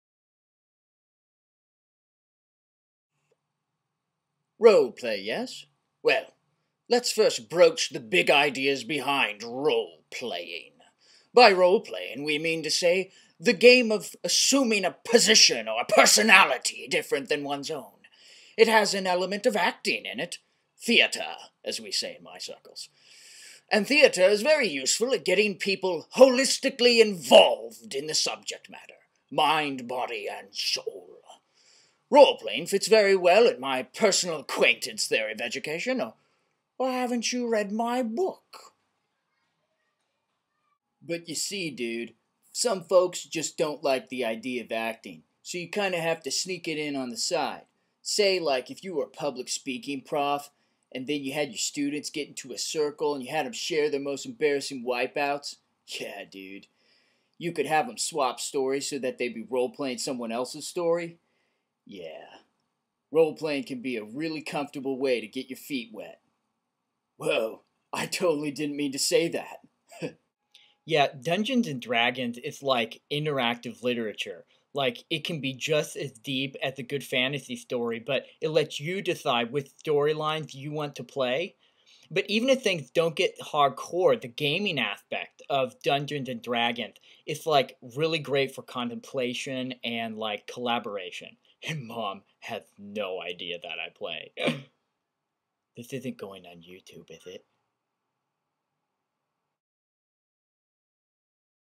Role Playing Podcast - Ryan Swindoll
A brief glance at role playing through the eyes of three stereotypes.